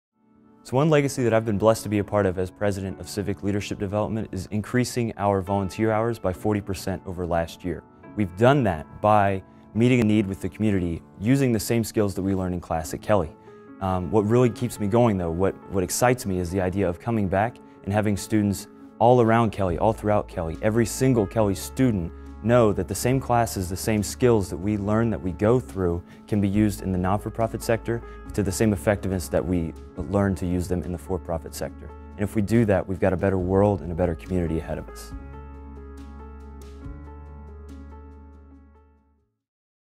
My Kelley Legacy Adam Putnick, BS'18
Kelley School of Business student Adam Putnick hopes his legacy will inspire future generations of Kelley students.